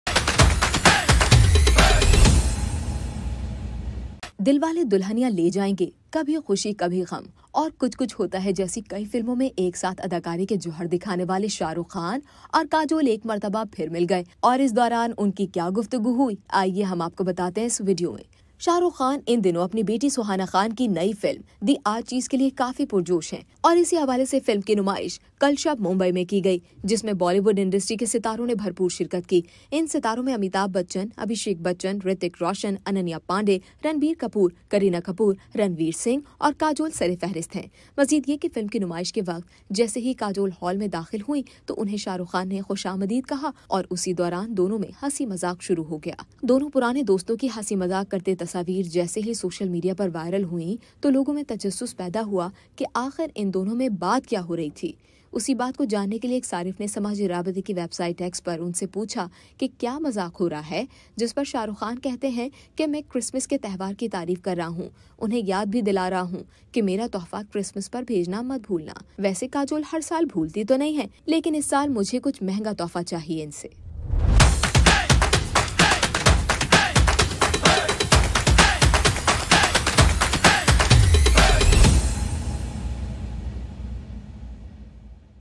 Shahrukh Khan And Kajol Became The Centre Of Attention At The Archies Premiere | Hungama Express
Shahrukh Khan And Kajol Became The Centre Of Attention At The Archies Premiere